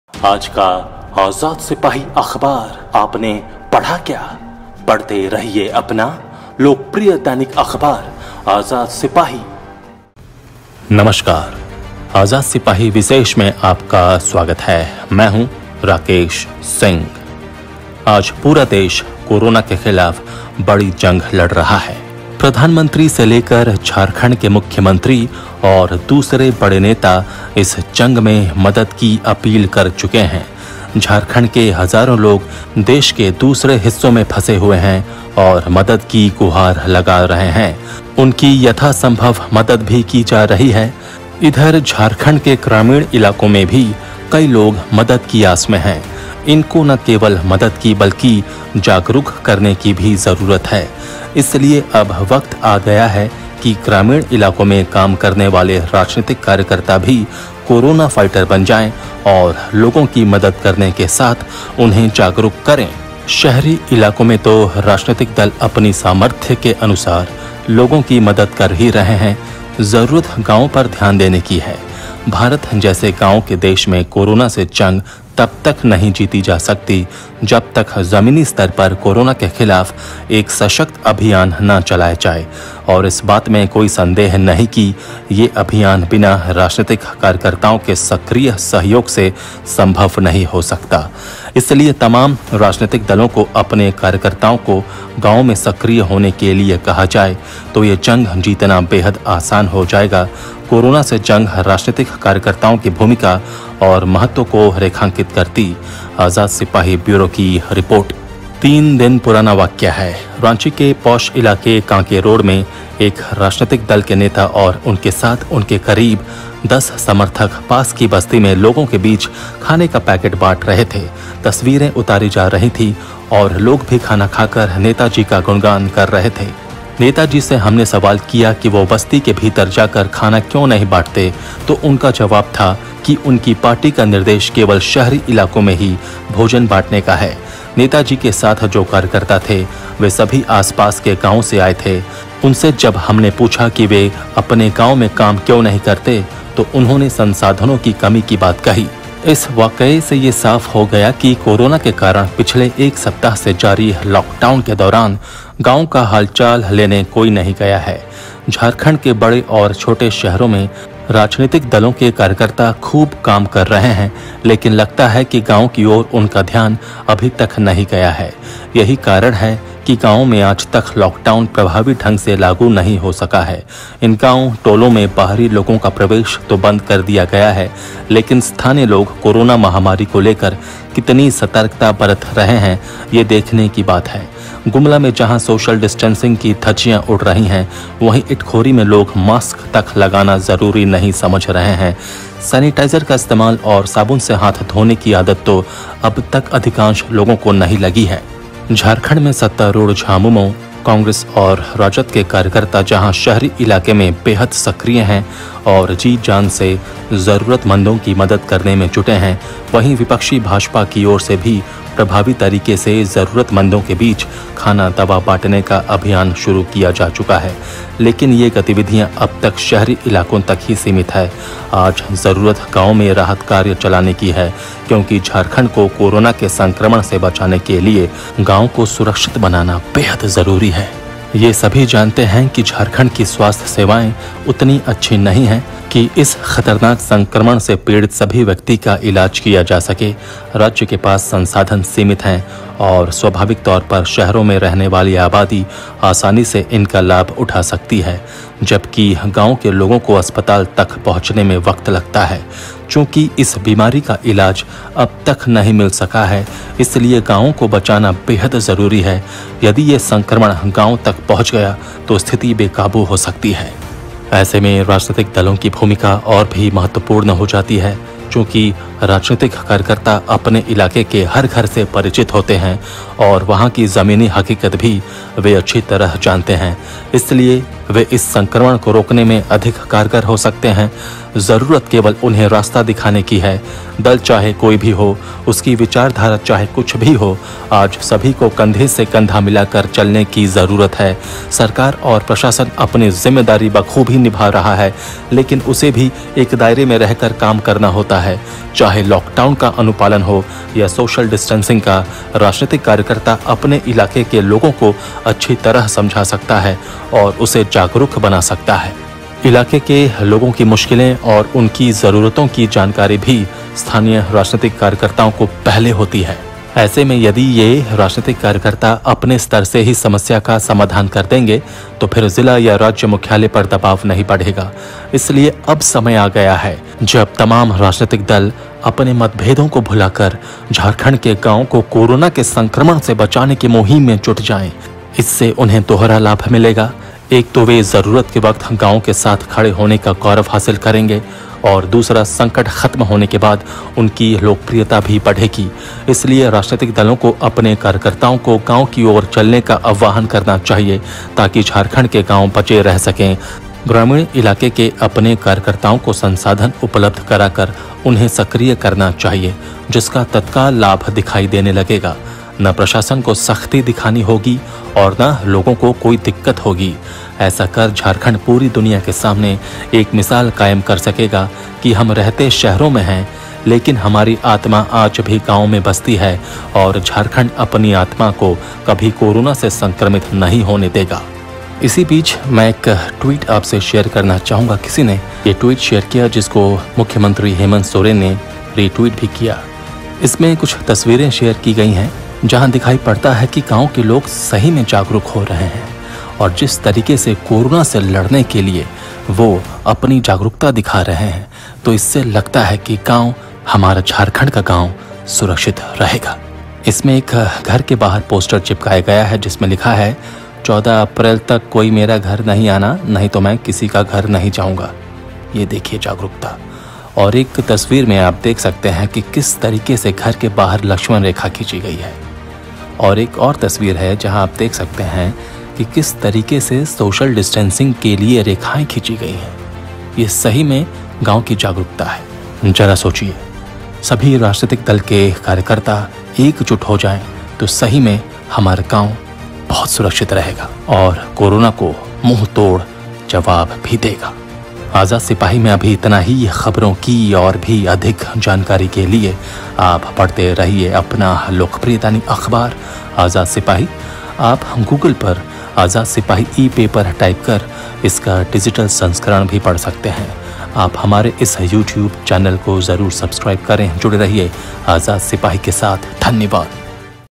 हमारे झारखंड का गांव तभी कोरोना को मुँह तोड़ जवाब देगा | आजाद सिपाही विशेष
लॉकडाउन को सफल बनाने चलें गांव की ओर | आजाद सिपाही विशेष